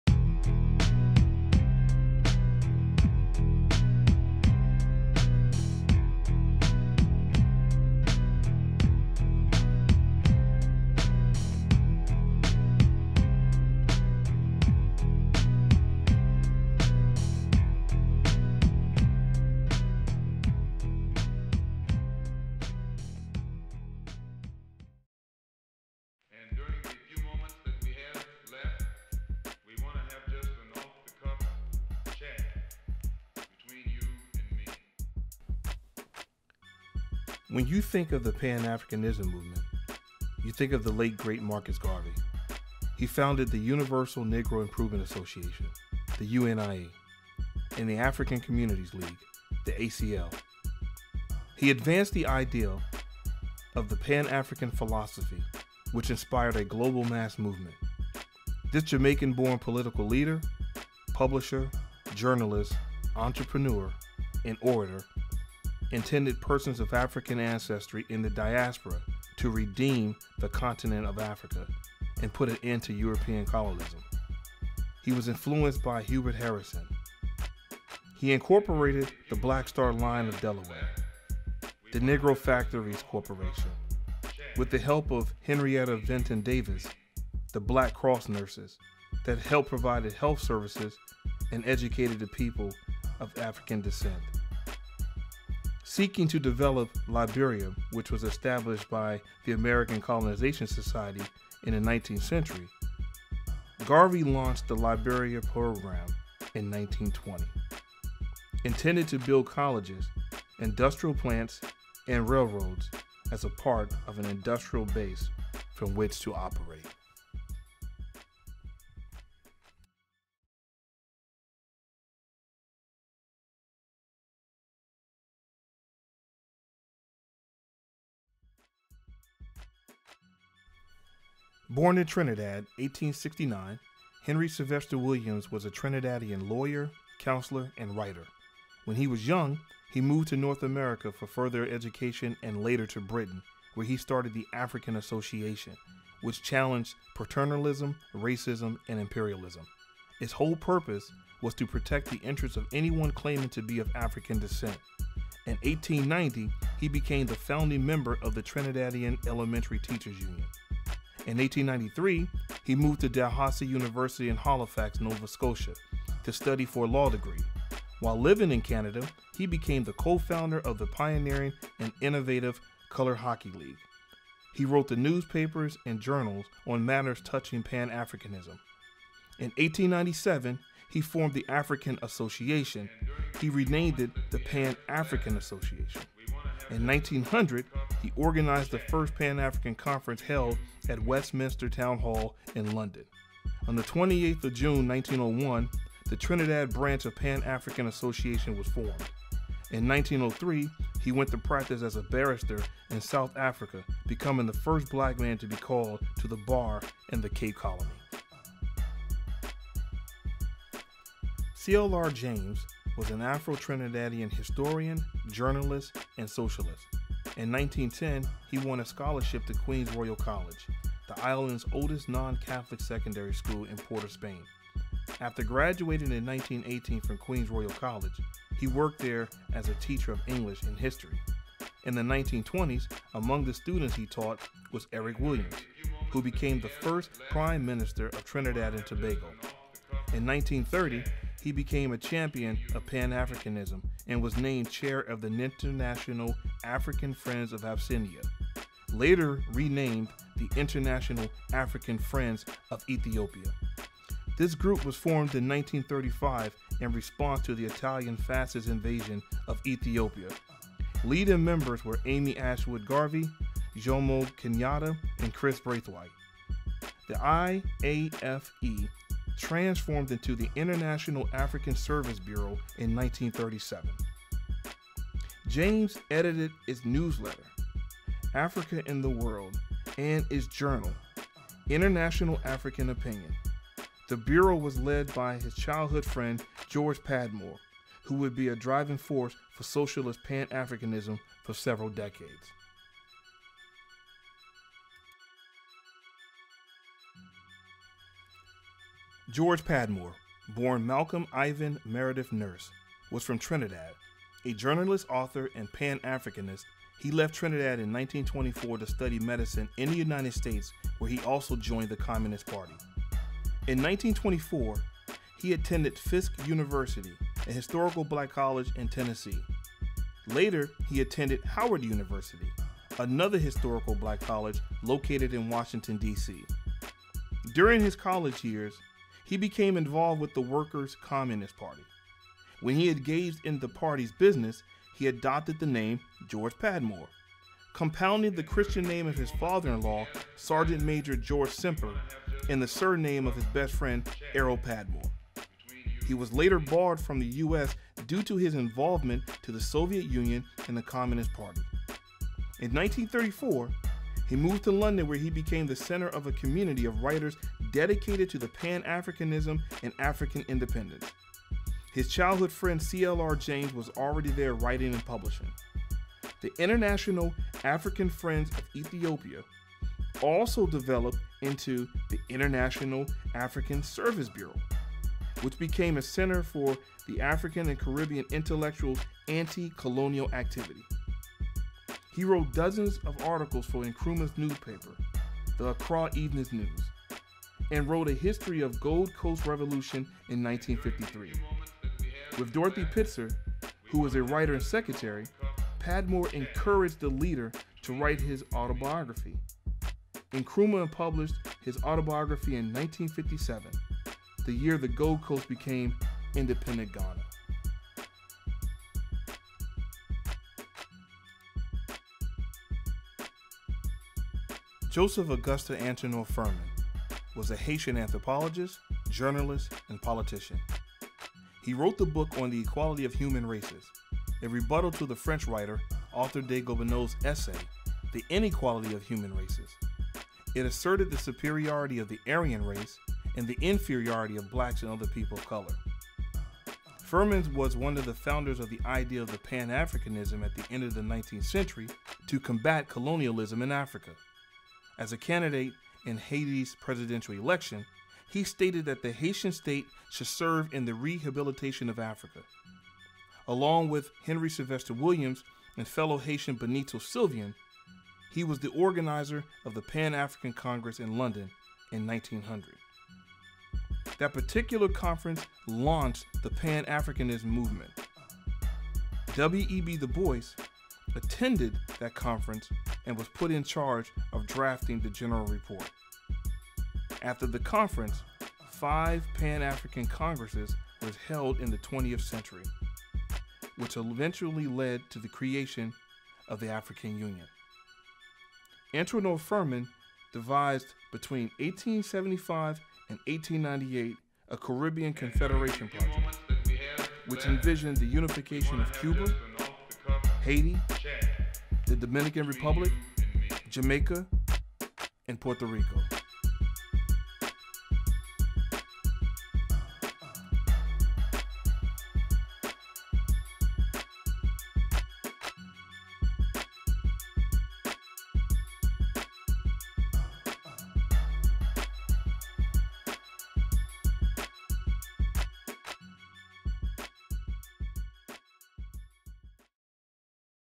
The Heads of Pan-Africanism
Learn about the founders of the Pan African movement and their contributions.
Pan-Africanism is a worldwide movement that aims to encourage and strengthen bonds of solidarity between all people of African descent